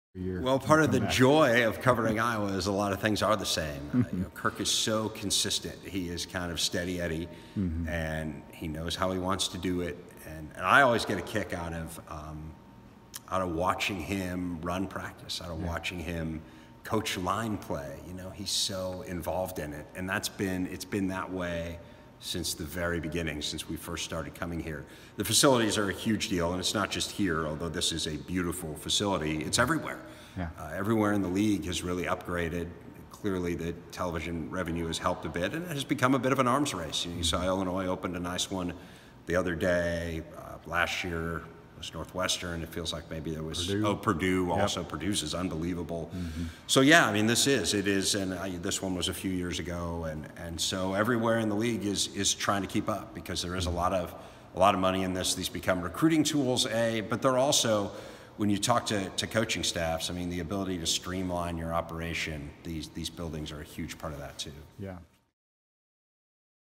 The thing Big Ten Network's Dave Revsine loves about coming to Iowa
Revsine has been the Big Ten Network's studio host since its 2007 launch and the tour has been officially in motion for 11 years.